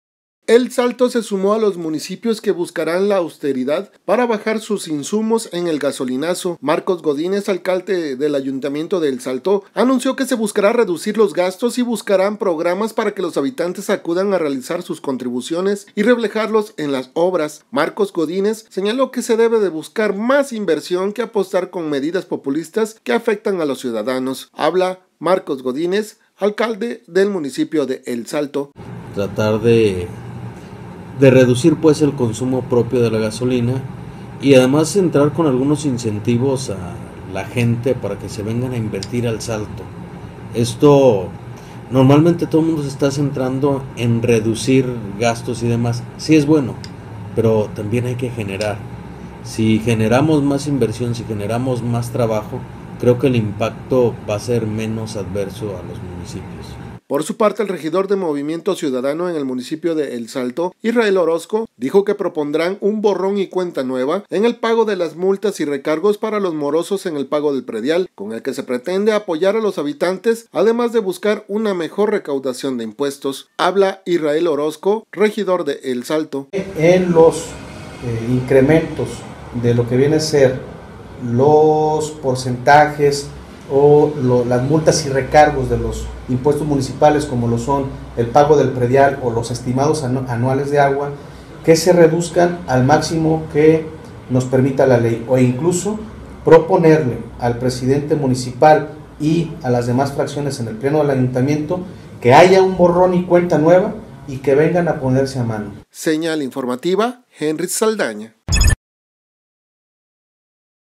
El Salto se suma a los municipios que toman medidas contra el gasolinazo
Marcos Godínez anunció que se buscará reducir gastos y promover que los ciudadanos acudan a pagar sus contribuciones.